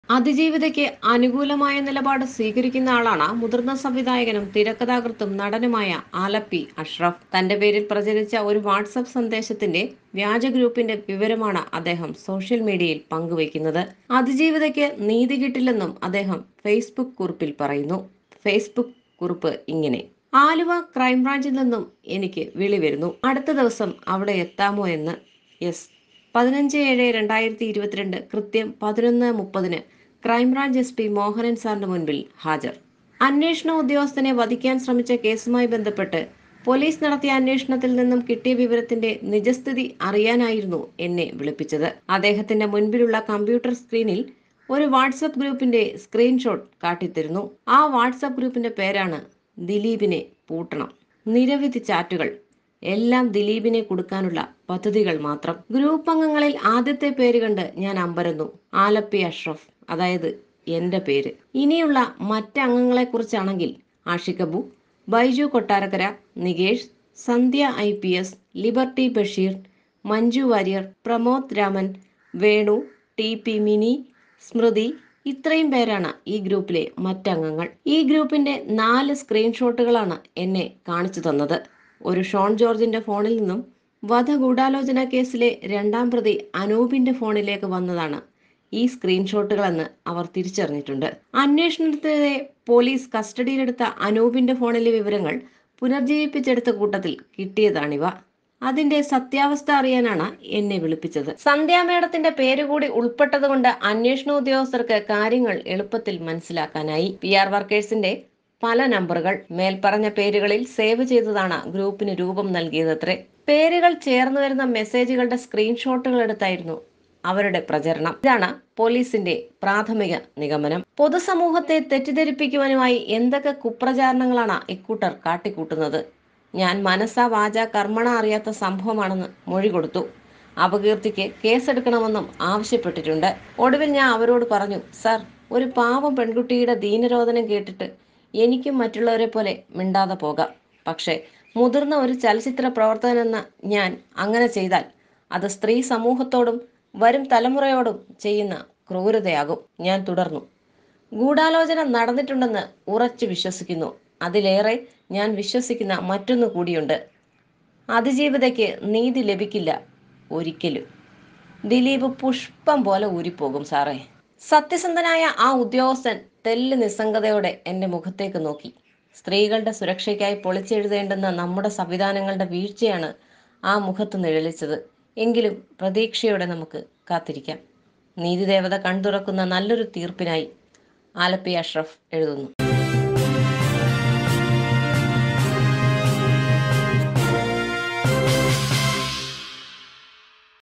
ദിലീപ് പുഷ്പം പോലെ ഊരിപോകും... ആലപ്പി അഷറഫിന്റെ ഫേസ്ബുക്ക് പോസ്റ്റ് | Dileep Alleppey Ashraf
അതിജീവിതയ്ക്ക് അനുകൂലമായ നിലപാട് സ്വീകരിക്കുന്നയാളാണ് മുതിര്‍ന്ന സംവിധായകനും തിരക്കഥാകൃത്തും നടനുമായ ആലപ്പി അഷറഫ്. തന്റെ പേരില്‍ പ്രചരിച്ച ഒരു വ്യാജ വാട്‌സാപ്പ് സന്ദേശത്തിന്റെ വ്യാജ ഗ്രൂപ്പിന്റെയും വിവരമാണ് അദ്ദേഹം സോഷ്യല്‍ മീഡിയയില്‍ പങ്കുവയ്ക്കുന്നത്. അതിജീവിതയ്ക്ക് നീതി കിട്ടില്ലെന്നും അദ്ദേഹം ഫേസ്ബുക്ക് കുറിപ്പില്‍ പറയുന്നു.

Veteran director, screenwriter and actor Alleppey Ashraf is a supporter of the survivor,  He is also sharing the information of a fake WhatsApp message circulated in his name and a fake group on social media. He says in a Facebook note that the survivor will not get justice.

This video is related to the following topics:
dileep,
alleppey ashraf,
kavya madhavan,
kerala police,